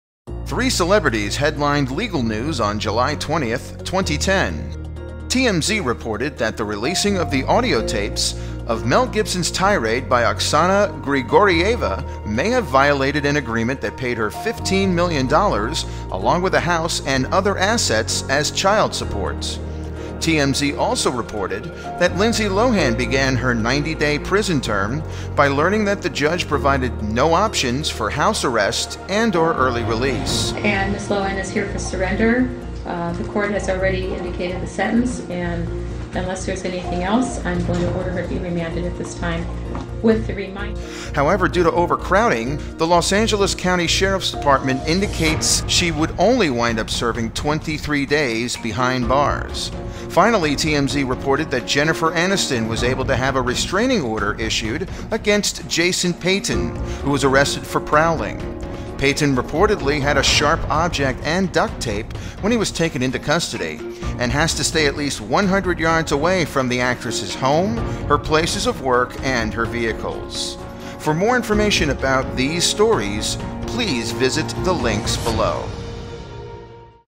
Mel Gibson/Grigorieva $15 Million; Lindsay Lohan begins prison; Jennifer Aniston stalker
Three celebrities headlined legal news on July 20th, 2010.  TMZ reported that the releasing of the audio tapes of the Mel Gibson tirade by Oksana Grigorieva may have violated an agreement that paid her $15 million dollars, along with a house and other assets as child support.

TMZ also reported that Lindsay Lohan began her 90-day prison term by learning that the judge provided no options for house arrest and/or early release. However, due to overcrowding, TMZ further reports the Los Angeles County Sheriff's Department indicated that she would likely wind up serving only 23 days behind bars.

Finally, TMZ reported that Jennifer Aniston was able to have a restraining order issued against Jason Peyton, who was arrested for prowling. Peyton reportedly had a sharp object and duct tape when he was taken into custody, and has to stay at least 100 yards away from the actress's home, her places of work, and her vehicles.